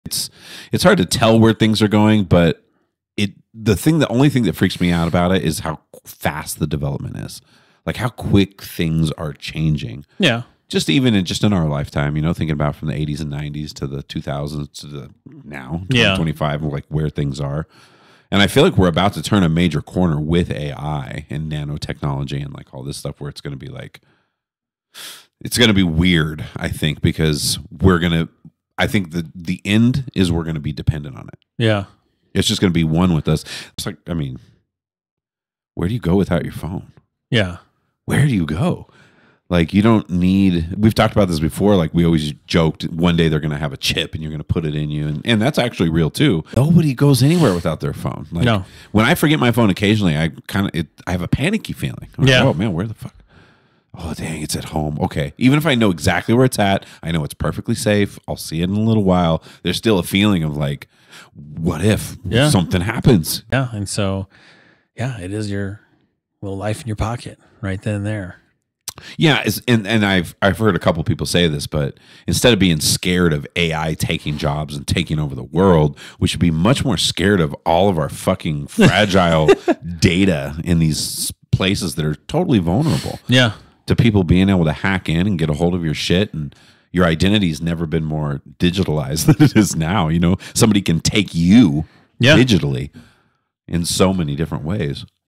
AI & Future Tech: Our Dependence and Data Fears
AI & nanotechnology are rapidly changing. Are we turning a major corner? The future may be total dependence. What happens when our fragile data is vulnerable?